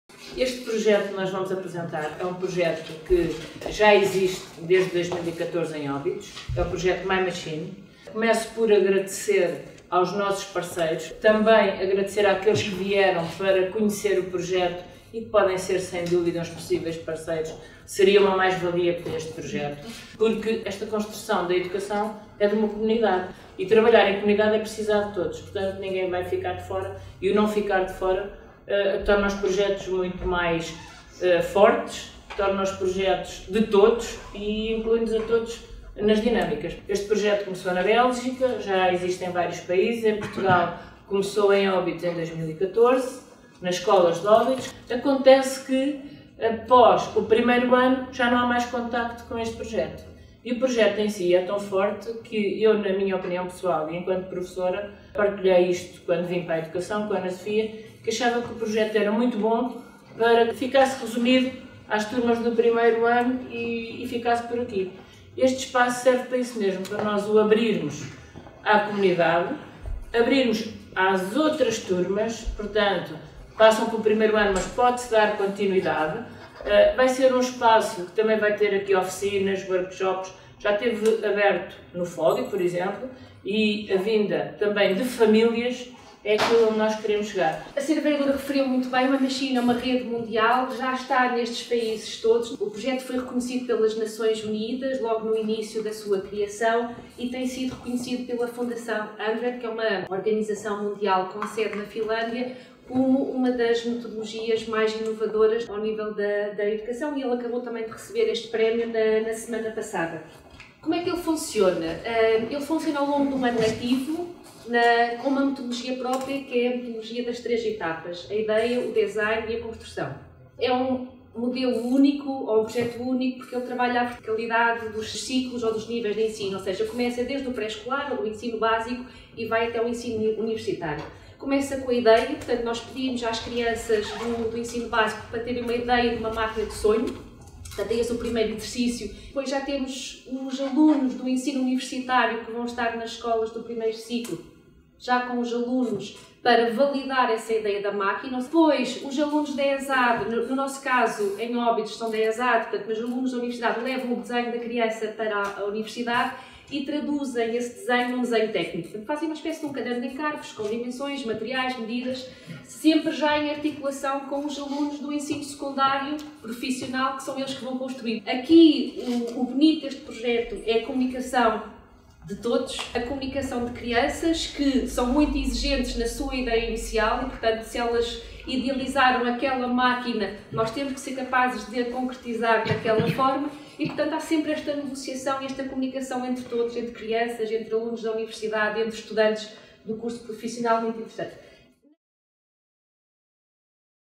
Inauguração do Espaço MyMachine Portugal em Óbidos
Inauguração do Espaço MyMachine Portugal, no dia 11 de Novembro de 2022, a funcionar na Casa do Centro (antiga biblioteca municipal), em Óbidos.
Em Óbidos, o projeto reúne o Município de Óbidos, o Agrupamento de Escolas Josefa de Óbidos, o Parque Tecnológico de Óbidos, a Escola Superior de Artes e Design das Caldas da Rainha, do Instituto Politécnico de Leiria, o CENFIM – núcleo das Caldas da Rainha e o Centro de Educação Especial Rainha D. Leonor, das Caldas da Rainha. Foi ainda celebrado um novo protocolo com o Agrupamento de Escolas Rafael Bordalo Pinheiro, das Caldas da Rainha.